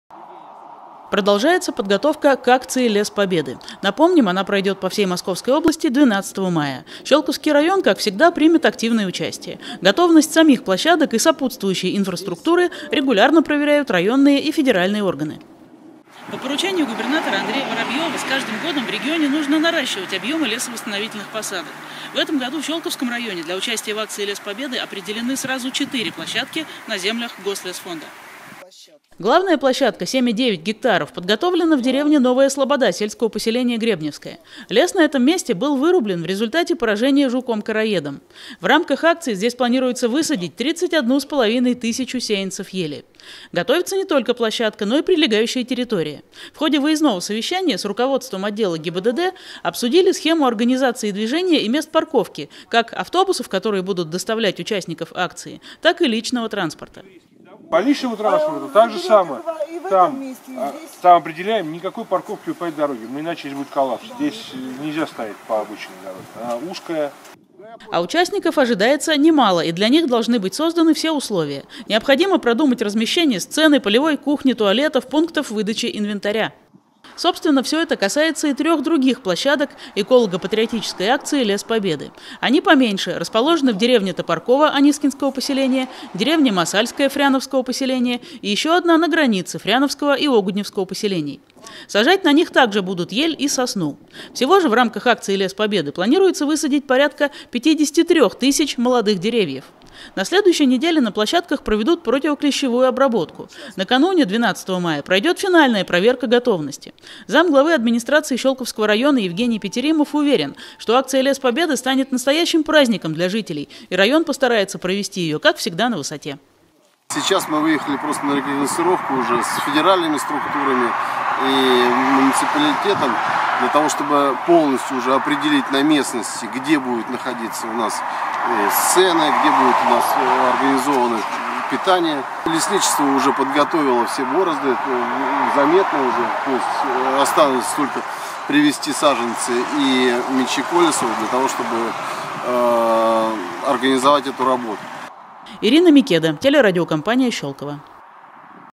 Готовимся сажать Лес Победы
Продолжается подготовка к акции Лес Победы. Она пройдёт по всей Московской области, в том числе в Щёлковском районе, 12 мая. Готовность самих площадок и сопутствующей инфраструктуры регулярно проверяют районные и федеральные органы.